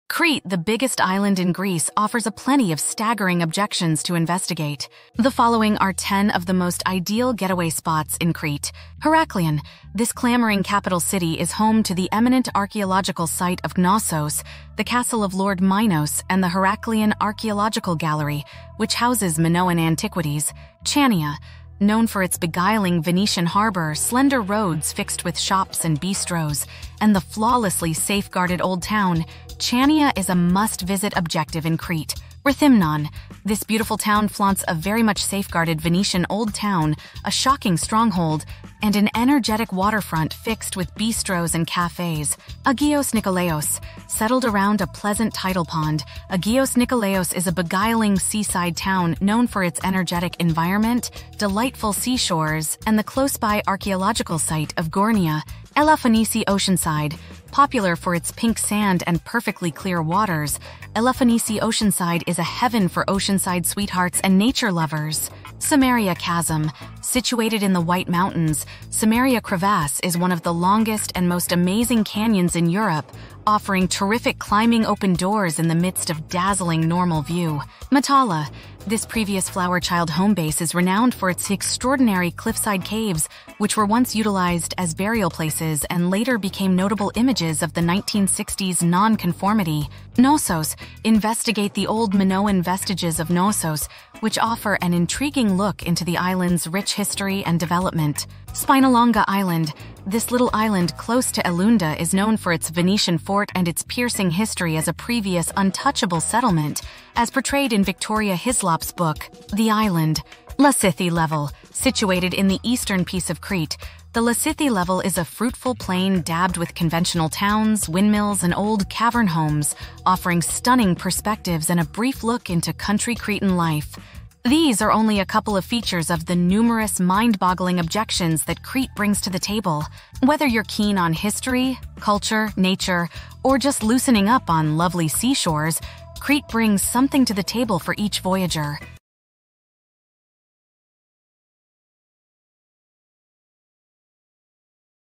10 Best Places to Visit in Crete
Crete, the largest island in Greece, offers a plethora of stunning destinations to explore. Here are ten of the best places to visit in Crete:

    Heraklion: This bustling capital city is home to the renowned archaeological site of Knossos, the Palace of King Minos, and the Heraklion Archaeological Museum, which houses Minoan artifacts.

    Chania: Known for its charming Venetian harbor, narrow streets lined with shops and cafes, and the beautifully preserved old town, Chania is a must-visit destination in Crete.

    Rethymnon: This picturesque town boasts a well-preserved Venetian old town, a stunning fortress, and a lively waterfront lined with cafes and restaurants.

    Agios Nikolaos: Nestled around a picturesque lagoon, Agios Nikolaos is a charming coastal town known for its vibrant atmosphere, beautiful beaches, and the nearby archaeological site of Gournia.

    Elafonisi Beach: Famous for its pink sand and crystal-clear waters, Elafonisi Beach is a paradise for beach lovers and nature enthusiasts.

    Samaria Gorge: Located in the White Mountains, Samaria Gorge is one of the longest and most breathtaking gorges in Europe, offering spectacular hiking opportunities amidst stunning natural scenery.

    Matala: This former hippie hangout is famous for its unique cliffside caves, which were once used as tombs and later became iconic symbols of the 1960s counterculture.

    Knossos: Explore the ancient Minoan ruins of Knossos, which offer a fascinating glimpse into the island's rich history and civilization.

    Spinalonga Island: This small island near Elounda is known for its Venetian fortress and its poignant history as a former leper colony, as depicted in Victoria Hislop's novel "The Island."

    Lasithi Plateau: Located in the eastern part of Crete, the Lasithi Plateau is a fertile plain dotted with traditional villages, windmills, and ancient cave dwellings, offering breathtaking views and a glimpse into rural Cretan life.

These are just a few highlights of the many incredible destinations that Crete has to offer. Whether you're interested in history, culture, nature, or simply relaxing on beautiful beaches, Crete has something to offer every traveler.